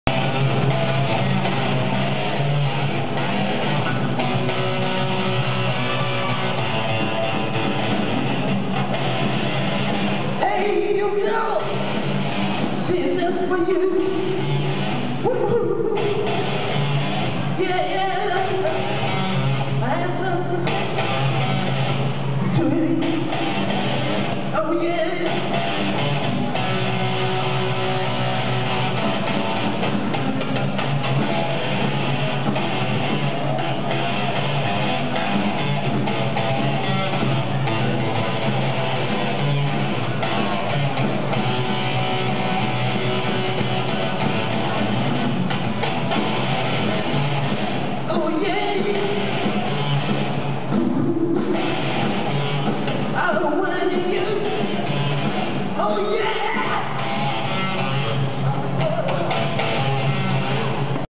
TRECHO SHOW LIBERHAS ESCOLA DA VILA
trecho do show da banda do oitavo ano da escola da vila
14/11/2009
josé abujamra e daniel nas guitarras e voz
mariana hope na bateria